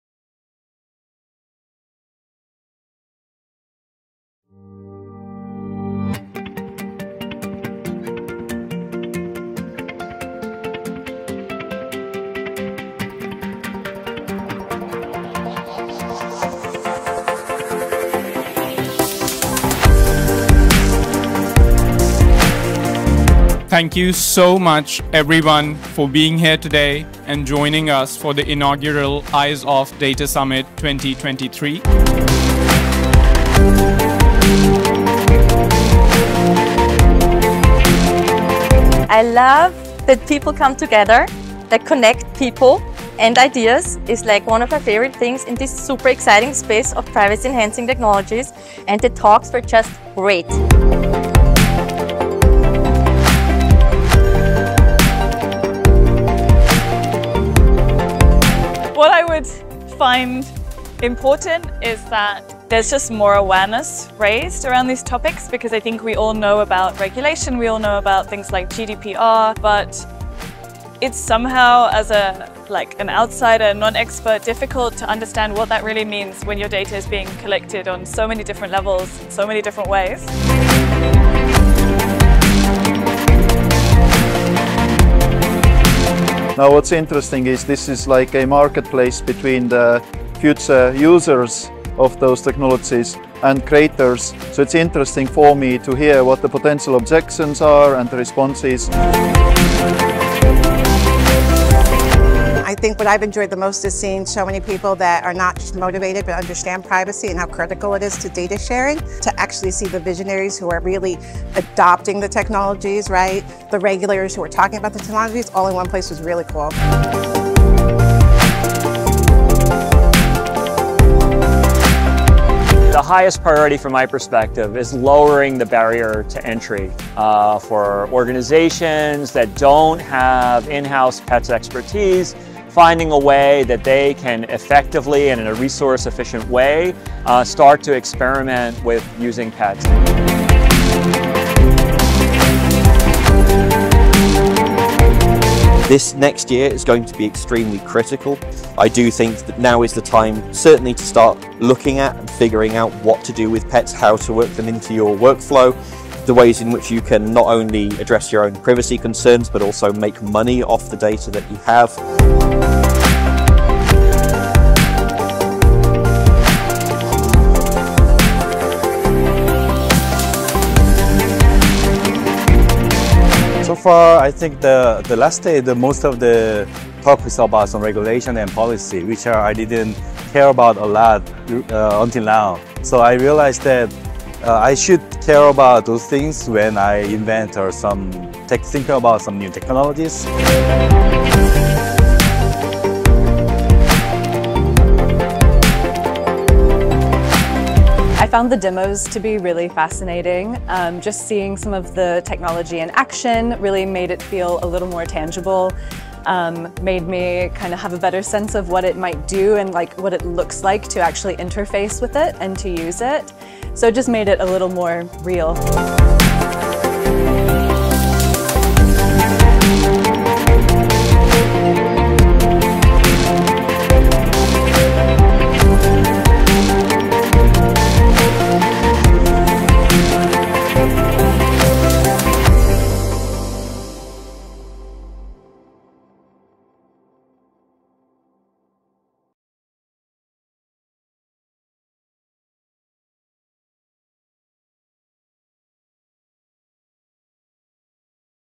Eyes-Off Data Summit 2023: Highlights & Speaker Insights
Relive the best moments from our Eyes-Off Data Summit held in Dublin on July 20-21. Dive into the key takeaways, candid moments, and insights shared by our speakers from across the globe.

Featuring insights from:

Katharina Koerner, Tech Diplomacy Network & AI Education Network
Morganne Graves, University of Oxford
Meelis Lootus, ML Technologies LTD & IEEE
Marcella Arthur, Inpher
Salil Vadhan, Harvard & OpenDP
Joseph Wilson, Optalysys
Kyoohyung Han, Samsung SDS
June Brawner, Royal Society

A huge thank you to Mark Harrison and the team at Harrison Media for creating such an awesome video